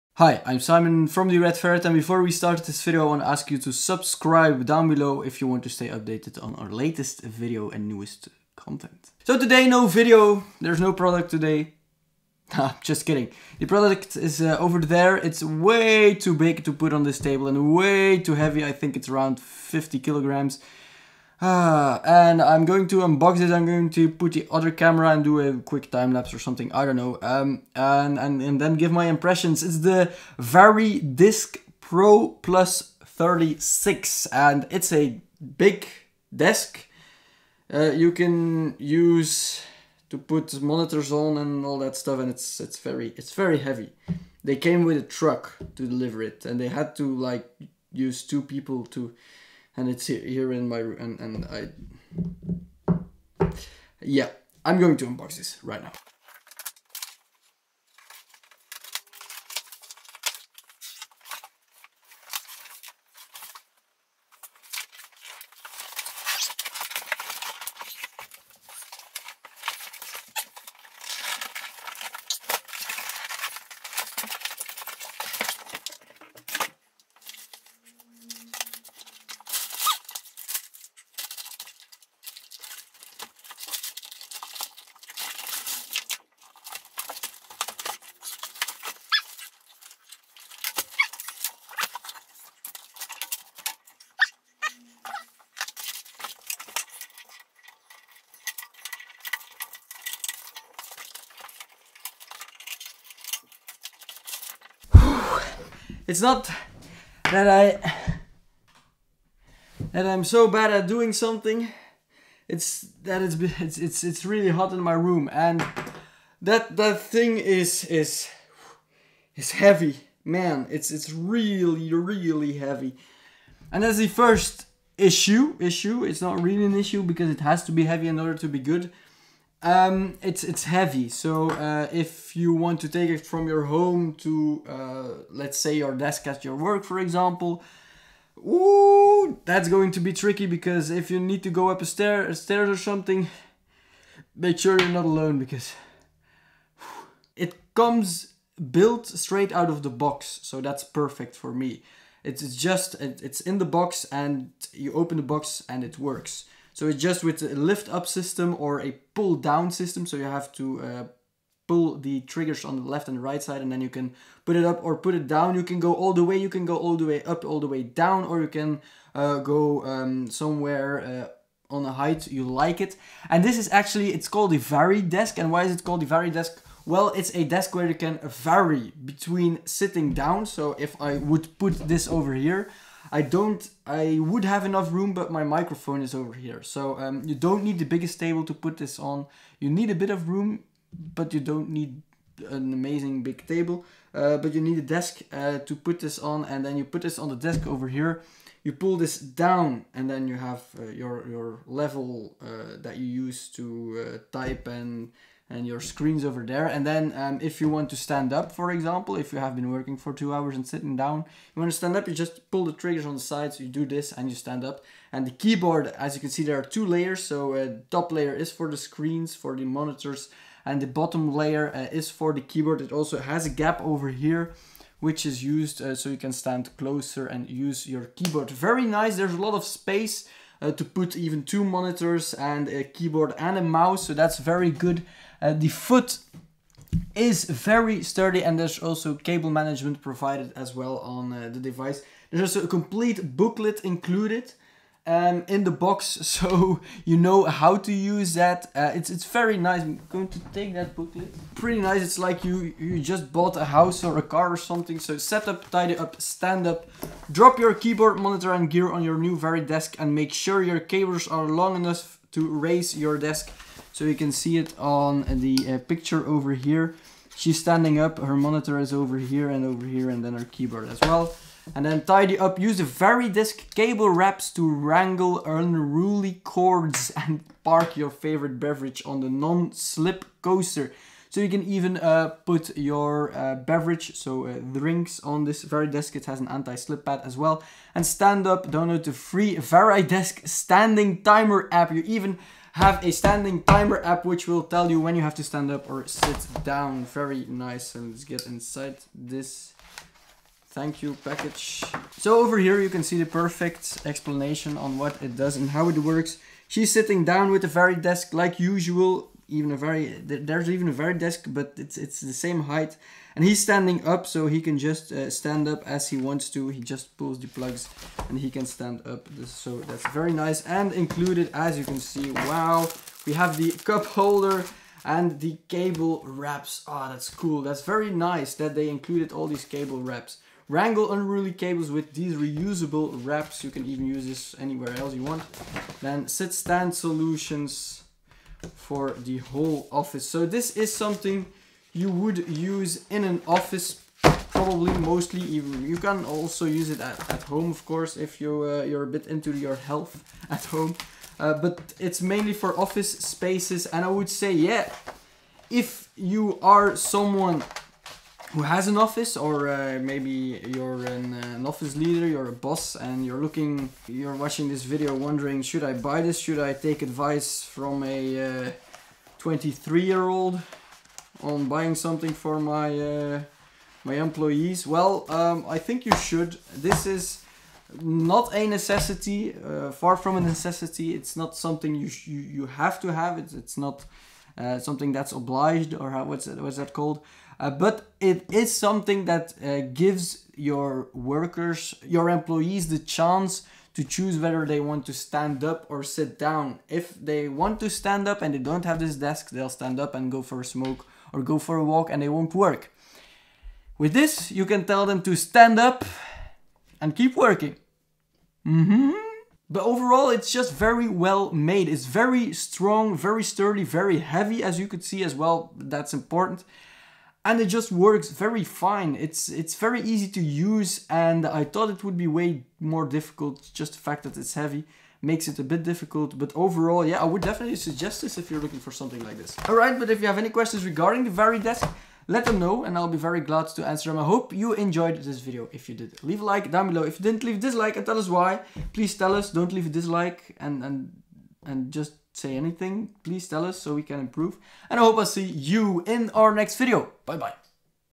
Varidesk Pro Plus 36 - You NEED to See this AMAZING Desk! [REVIEW]
The Varidesk is not a necessity, but it's awesome if you have one. It's amazingly sturdy and heavy, which makes it difficult to transport, but once it's in place you'll love it. It very easy to use (it also comes built out of the box) and you just pull the tabs on the side to put it higher or lower. It has a lot of accessories like cable management and a cup holder in the box, so if you're in need of a desk to stay healthy at work, the Varidesk might be the thing for you!